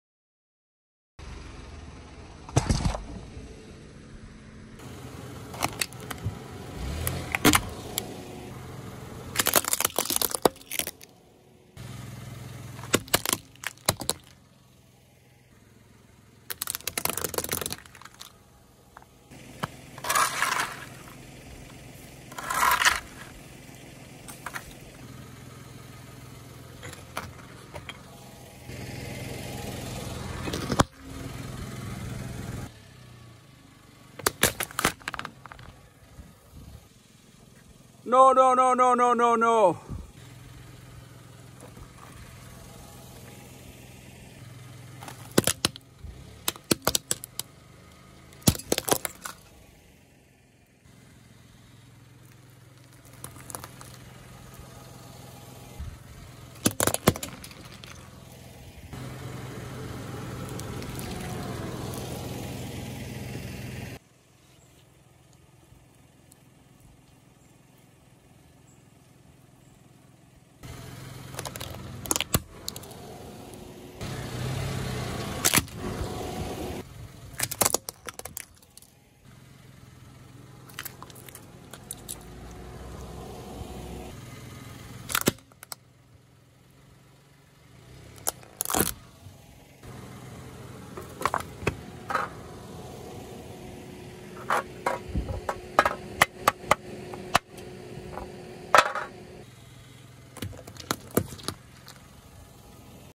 Crushing Crunchy & Soft Things by Car! Experiment Car Vs Candy
New experiments with cars create asmr. Car vs All
WARNING! Dangerous accident test, don't try this on your own !! Experiment : Car vs All
 ASMR: When this kind of annoying noise makes people ... sleepy With the goal of making sounds help you achieve relaxation.
We took inedible, expired and unused food to perform this experiment. See and you can say your comments in the comments Thanks a lot!